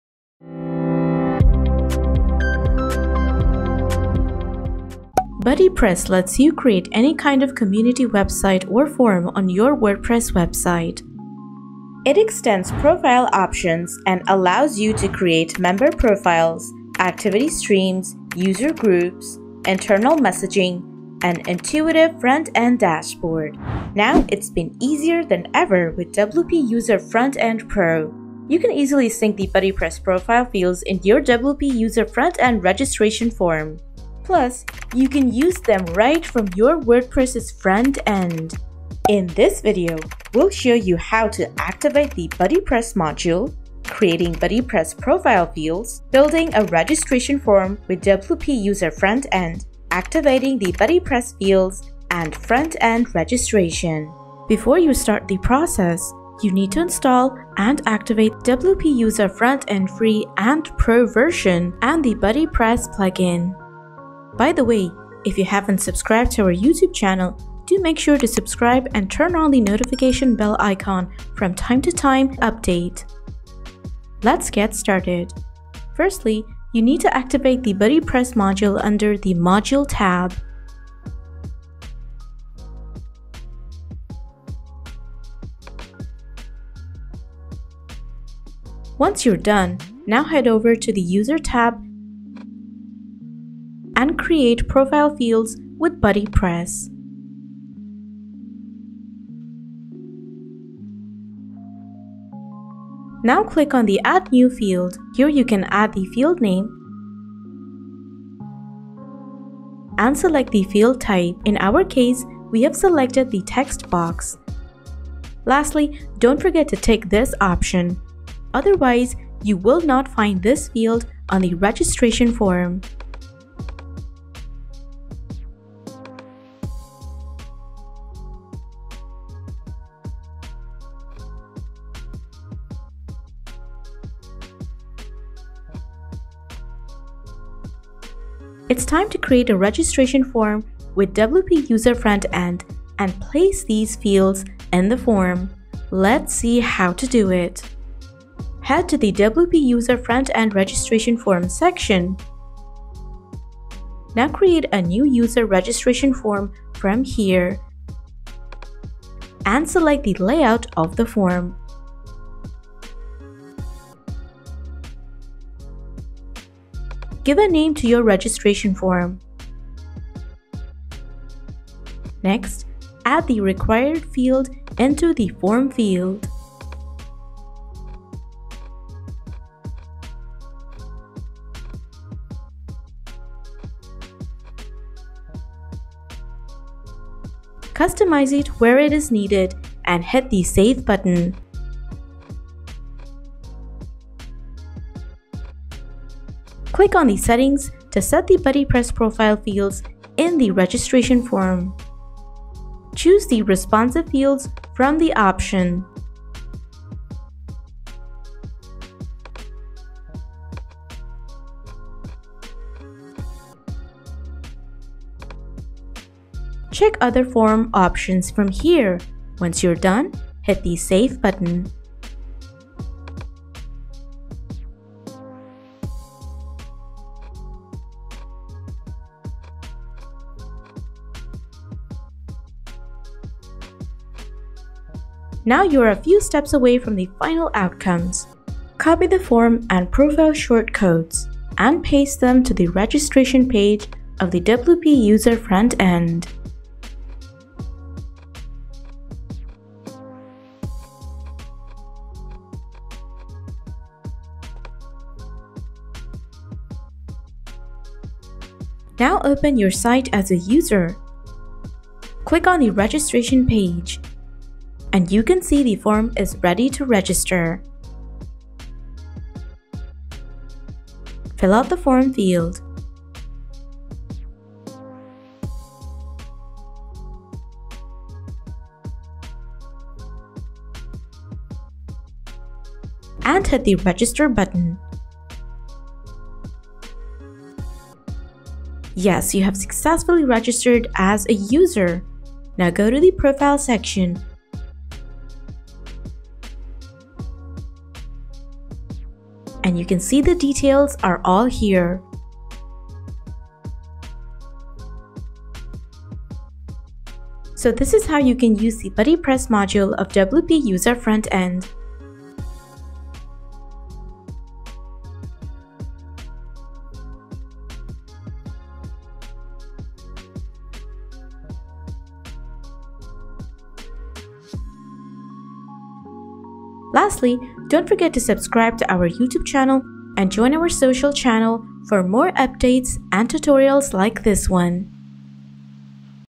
How to Add BuddyPress Module Profile Fields with WP User Frontend Pro
Want to set up BuddyPress profile fields and let users use them right away from the front end? Don't worry, we'll show you how to make BuddyPress profile fields easily and turn them on from the front end.

We all know that BuddyPress is a popular profile-building plugin built by WordPress itself. Also, you can create any kind of community website or forum on your WordPress website.

With the BuddyPress plugin, you can create:

01. Member profiles
02. Activity streams
03. User groups
04. Internal messaging
05. An intuitive frontend dashboard

The interesting fact is that you can use these fields with WP User Frontend Pro. You just have to follow these five steps:

01. Activate the BuddyPress Module
02. Create BuddyPress profile fields
03. Build a registration form with WPUF
04. Activate the BuddyPress fields
05. Frontend Registration by the Users

It’s a pro feature of the WP User Frontend. But you have to install and activate the free version before using the feature.

▶️ 0:00 Introduction
▶️ 0:04 Introducing BuddyPress Plugin
▶️ 0:28 Talking about the integration with BuddyPress and WP User Frontend
▶️ 0:42 Video outline
▶️ 1:18 Activating BuddyPress Module
▶️ 1:35 Creating BuddyPress Profile Fields
▶️ 2:29 Creating a Registration Form with WP User Frontend
▶️ 3:29 Activating the BuddyPress Module Profile Fields
▶️ 4:25 User Registration from the Frontend

Let’s check out the video tutorial to learn how to set up and customize the BuddyPress Module of WP User Frontend Pro on your WordPress site!

✦ Explore more tutorials on WP User Frontend